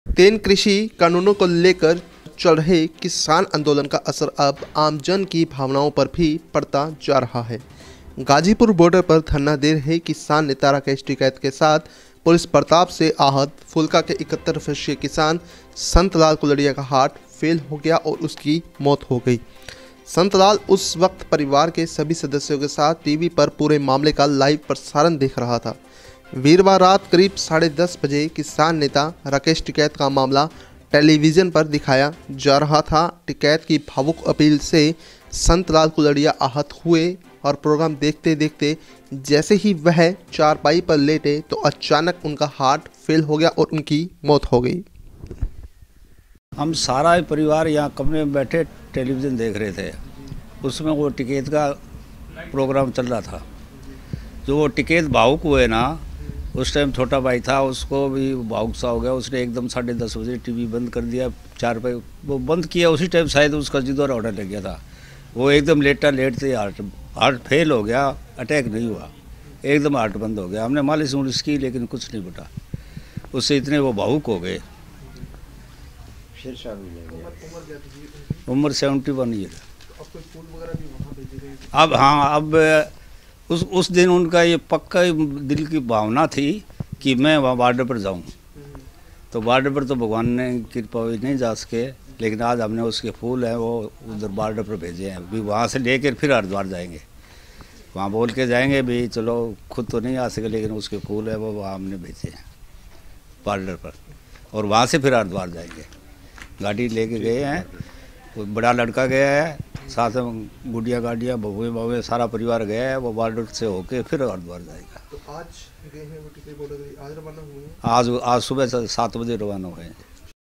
किसान नेता राकेश टिकैत का लाइव प्रोग्राम देख रहे सिरसा के किसान का हार्ट फेल, मौत
तीन कृषि कानूनों को लेकर दिल्ली की सीमाओं पर चल रहे किसान आंदोलन का असर अब आमजन की भावनाओंं पर पड़ता जा रहा है। गाजीपुर बार्डर पर धरना दे रहे किसान नेता राकेश टिकैत के साथ पुलिस बर्ताव से आहत फूलकां के 71 वर्षीय किसान संतलाल कुलडिय़ा का हार्ट फेल हो गया व उसकी मौत हो गई। संतलाल उस वक्त परिवार के सभी सदस्यों के साथ टीवी पर पूरे मामले का लाइव प्रसारण देख रहा था। वीरवार रात करीब साढ़े 10 बजे किसान नेता राकेश टिकैत का मामला दिखाया जा रहा था। टिकैत की भावुक अपील से संतलाल कुलडिय़ा आहत हुए। प्रोग्राम देखते देखते जैसे ही वह चारपाई पर लेटे तो  अचानक उनका हार्ट फेल हो गया।